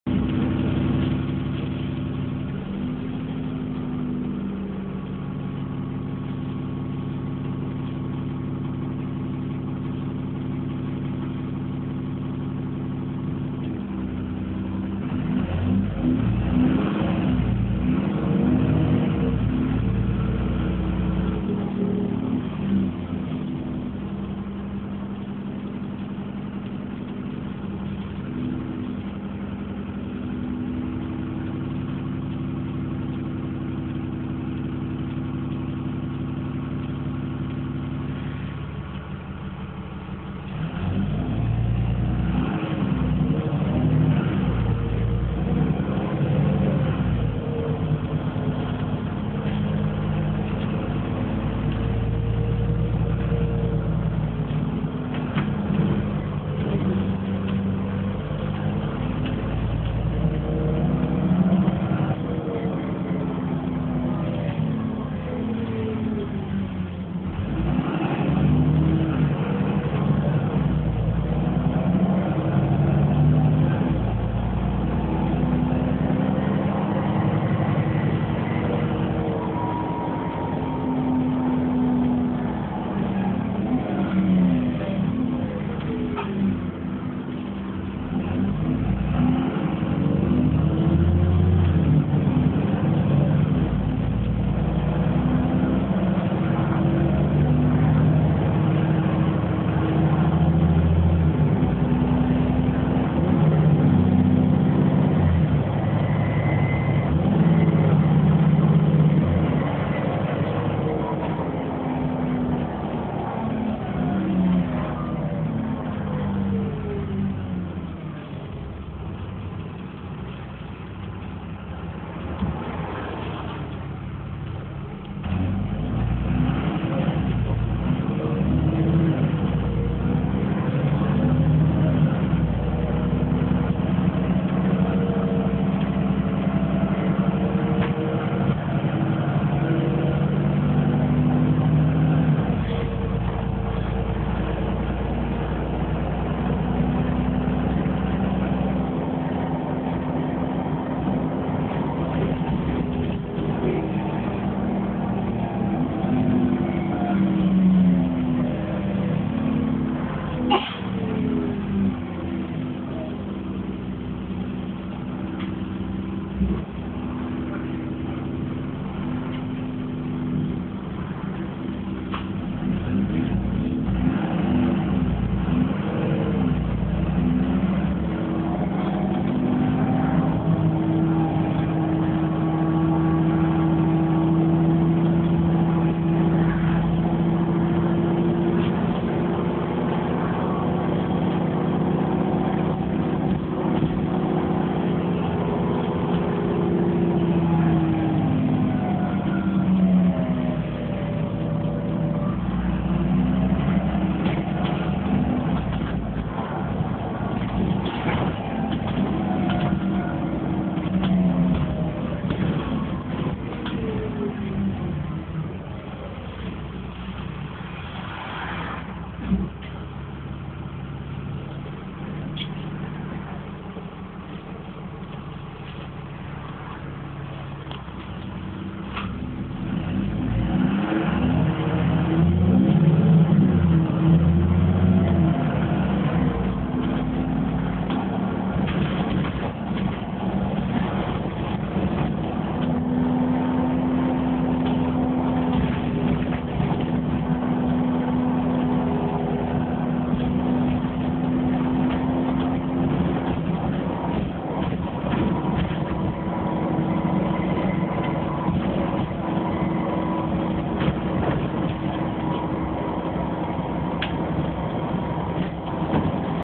Arriva Yorkshire Enviro400 1903
alexander dennis enviro400 taken from dewsbury to bradford on 268 first day in service.